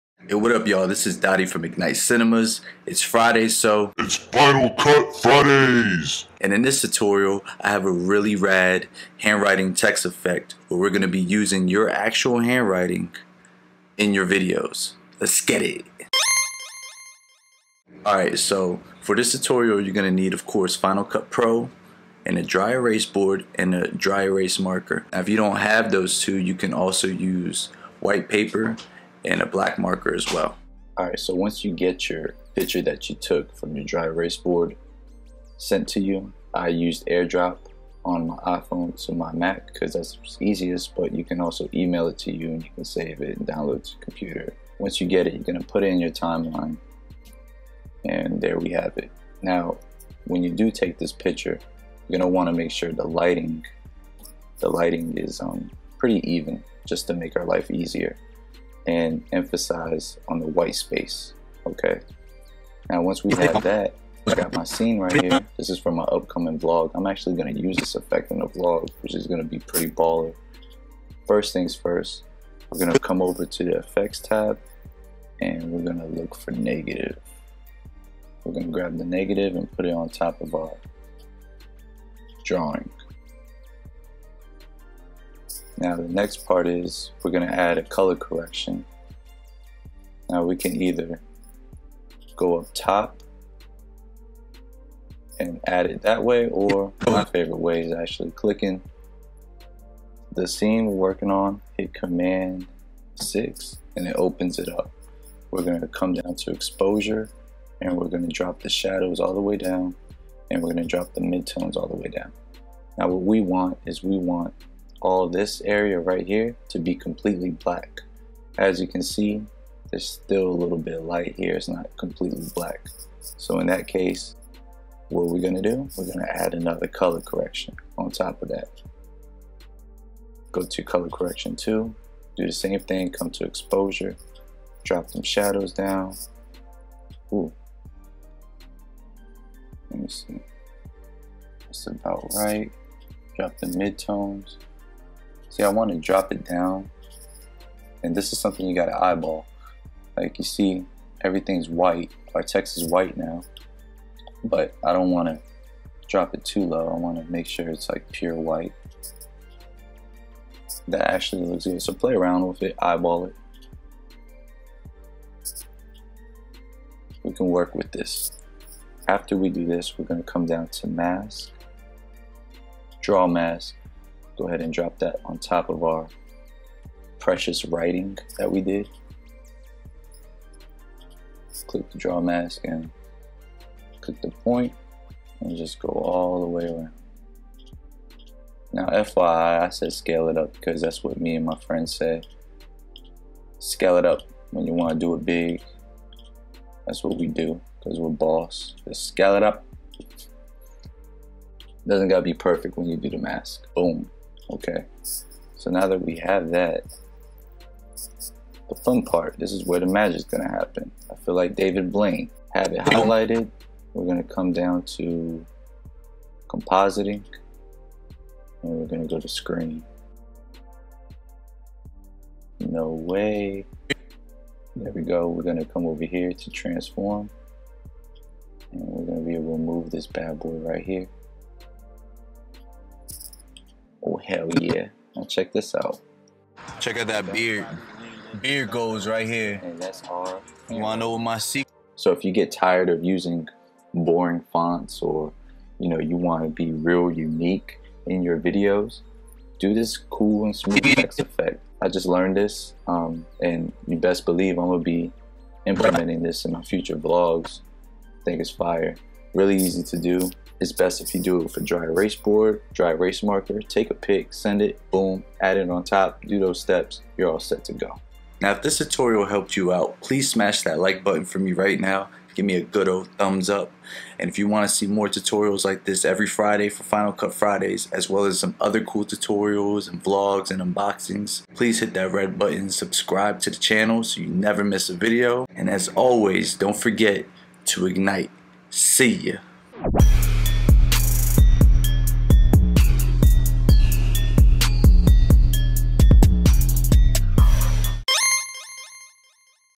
EASILY Make Handwritten Text In Final Cut Pro (Tutorial)
Tired of using those same ol' boring fonts? I Got you! On this Final Cut Friday, I teach you how to use your very own handwriting as a cool video text effect.

and don't forget to go out & IGNITE !

Equipment used:

Panasonic GH4 Camera:

JOBY Gorillapod SLR Zoom Flexible Tripod w/ Ballhead